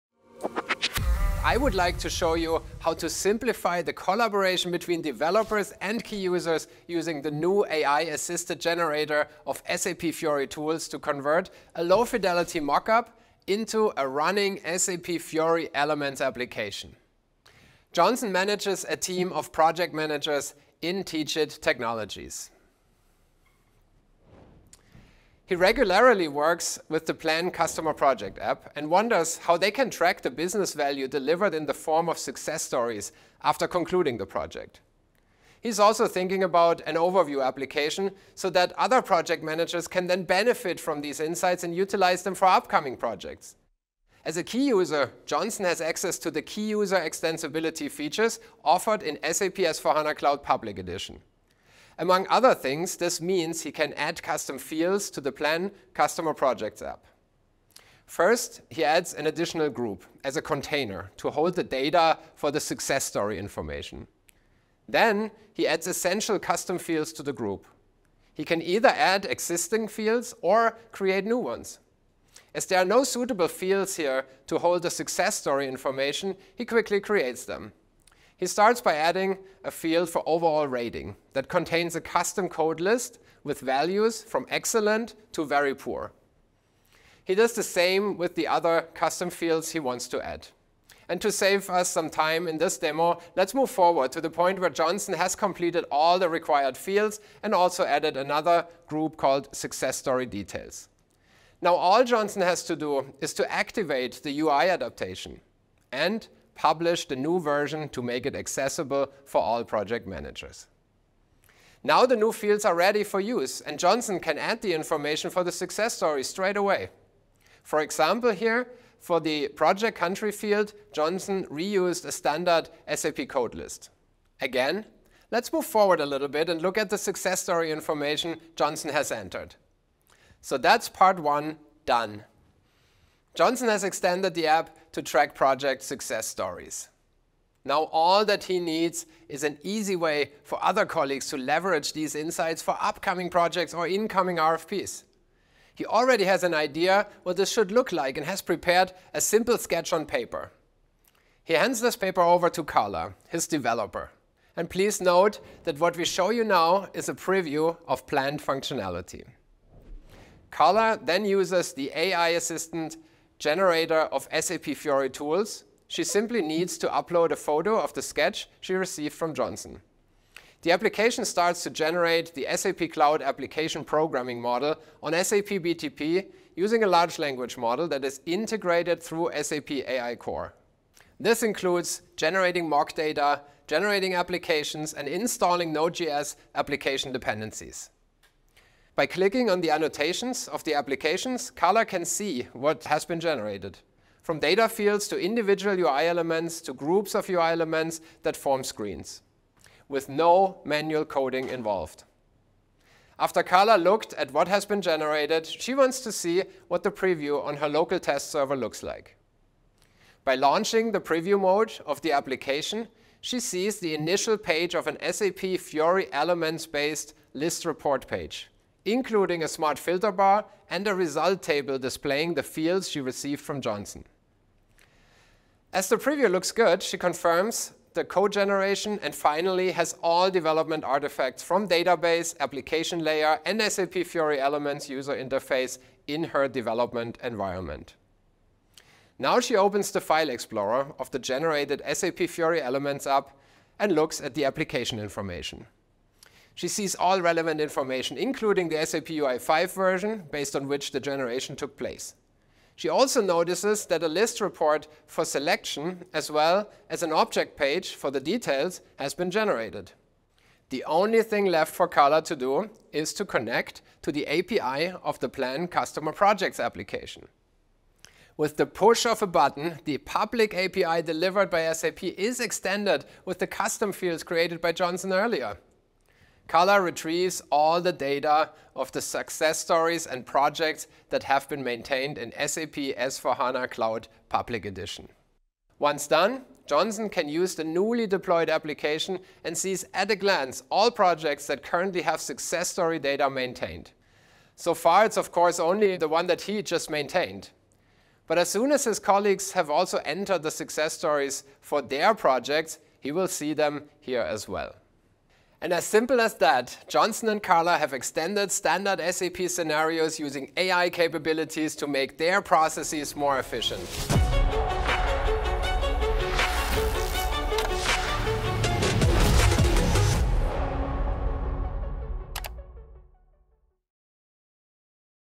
SAP Fiori Development with AI: Live Generator Demo
Experience the future of SAP Fiori app development with Jan Gilg as he showcases the groundbreaking AI-assisted generator in SAP Fiori tools.
Discover how to simplify developer and key user collaboration with SAP Fiori Tools' AI-assisted generator. Watch as we transform a low-fidelity mock-up into a fully functional SAP Fiori elements application, making project success tracking easier and more efficient.
Chapters:
    00:00 Introduction to AI-Assisted SAP Fiori Tools
    00:10 Johnson's Project Success Tracking Challenge
    00:40 Adding Custom Fields in SAP S/4HANA Cloud
    01:10 Activating and Publishing UI Adaptations
    01:40 Carla's AI-Driven Development Process
    02:10 Connecting and Extending APIs